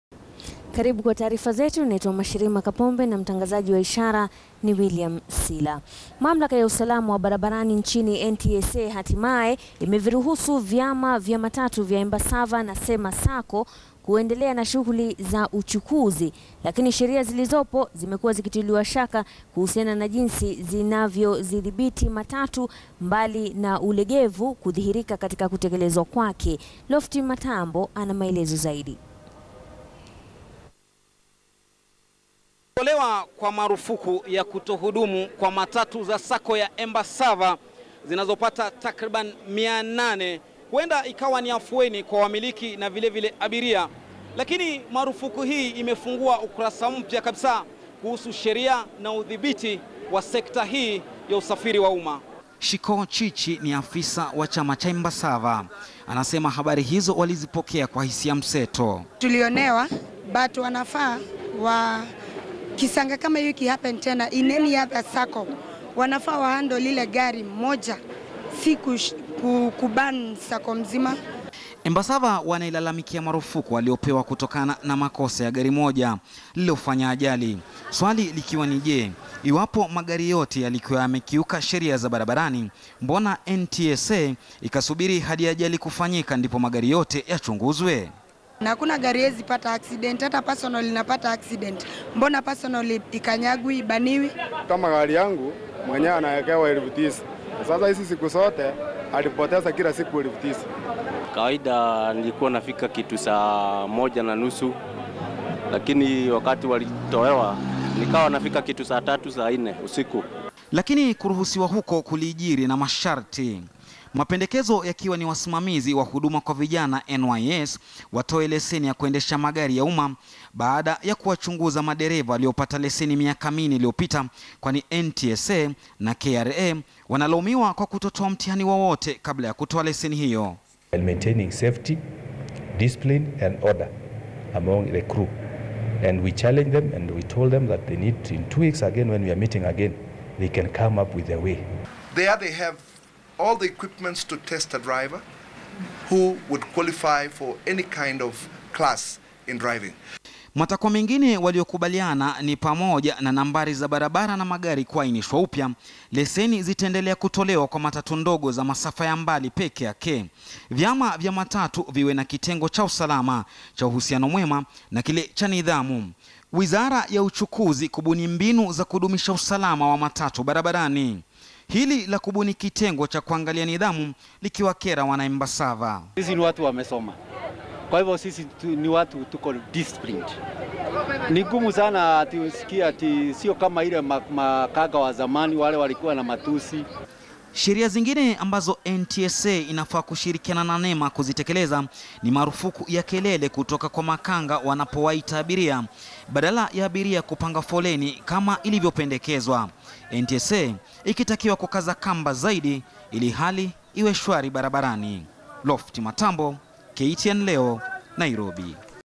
N.T.S.A yaruhusu vyama vya matatu vya Embassava na Sema Sacco kuendelea na shughuli za uchukuzi.
Mamlaka ya usalama wa barabarani nchini N.T.S.A hatimaye  imeviruhusu vyama vya matatu  vya Embassava na Sema sacco kuendelea na shughuli za uchukuzi. Lakini sheria zilizopo zimekuwa zikitiliwa shaka kuhusiana na jinsi zinavyozidhibiti matatu mbali na ulegevu kudhihirika katika kutekelezwa kwake. Lofty Matambo anaarifu.